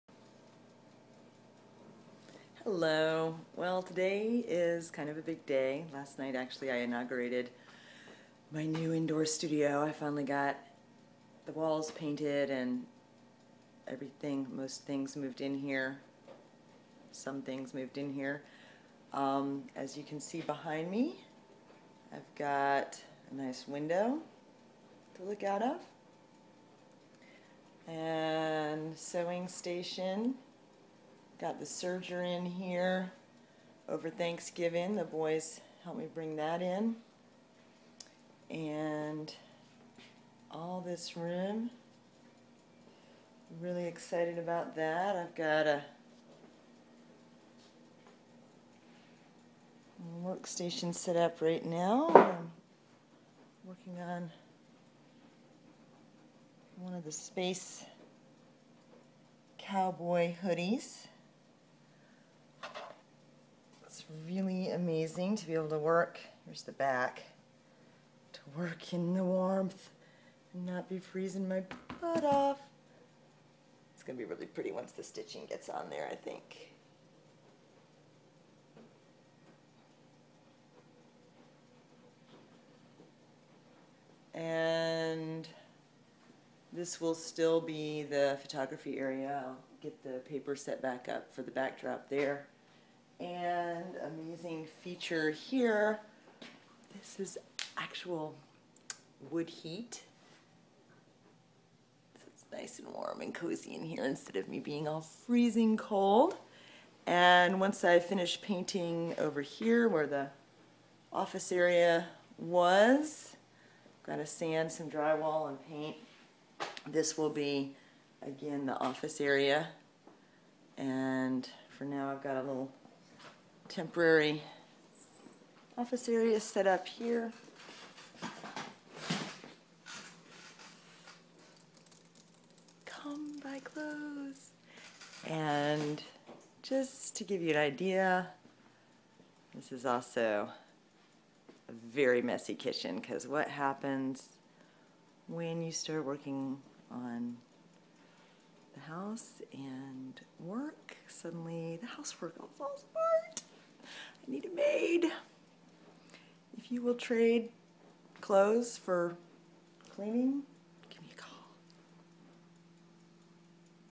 New studio space!
After last year in the unheated garage, (brrRRrRRrr!), I'm finally partially moved into the basement. A brief tour.